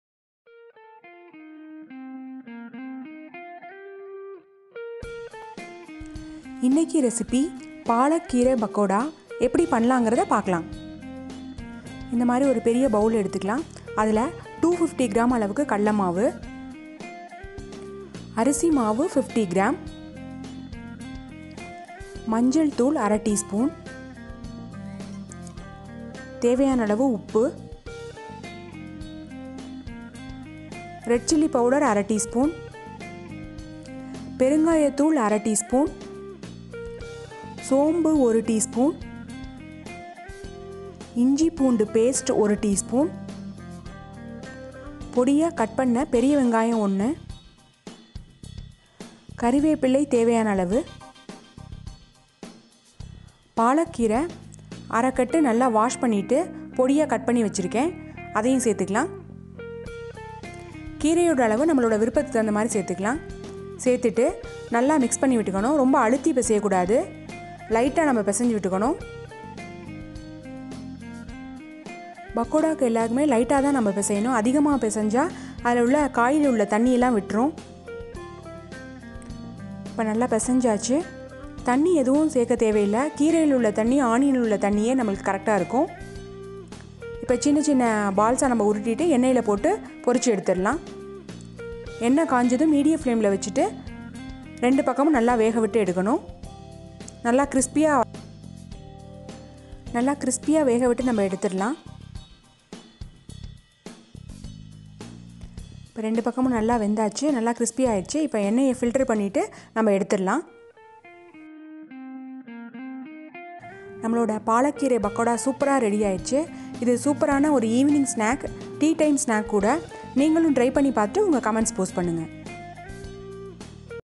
Palak Keerai Pakoda/ பாலக் கீரை பகோடா/Snack Recipe
Bala Balaji Kitchen

Evening snack recipe. easy to prepare tasty to eat

Ingredients

Gram flour-250gr

Rice flour-50gr

Turmeric powder-1/2tsp

Hing-1/2tsp

Fennal seeds-1tsp

Onion-1

Spinach Greens/Palak Keerai